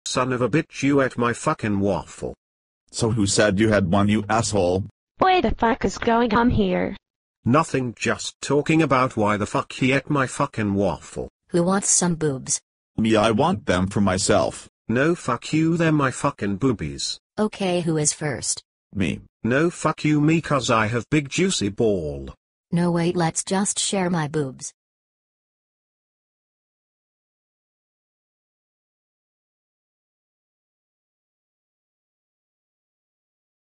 waffles
robot gets pissed because somebody ate his waffle